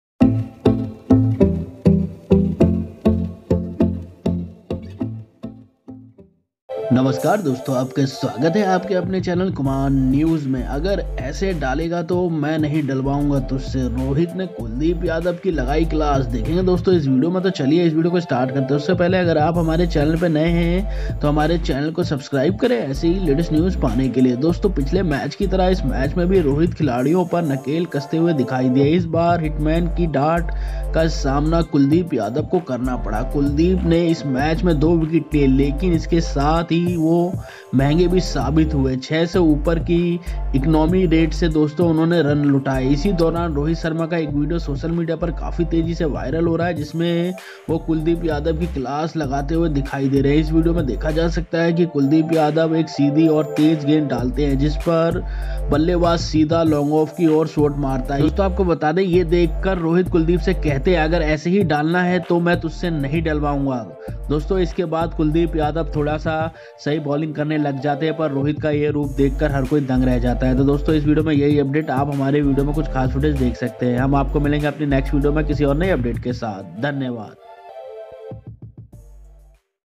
Rohit Sharma And Kuldeep Yadav Fight | Rohit Sharma Not Happy With Kuldeep Yadav Bowling
Video Highlights:::
In the third and last one-day match against West Indies, Team India performed brilliantly and won in a one-sided manner.  In this match too, Rohit Sharma did a great captaincy and did not let his fielders loose on any occasion.Like the previous match, in this match too Rohit was seen cracking down on the players and this time Kuldeep Yadav had to face the hitman's scolding.  Kuldeep took two wickets in this match but at the same time he proved to be expensive and wasted runs at an economy rate above 6.During this, a video of Rohit Sharma is also going viral in which he is seen taking Kuldeep's class.  It can be seen in this video that Kuldeep bowls a straight and fast delivery which the batsman shoots straight towards long off.  Seeing this ball, Rohit says to Kuldeep, 'If you want to put it like this, then I will not get it from you.'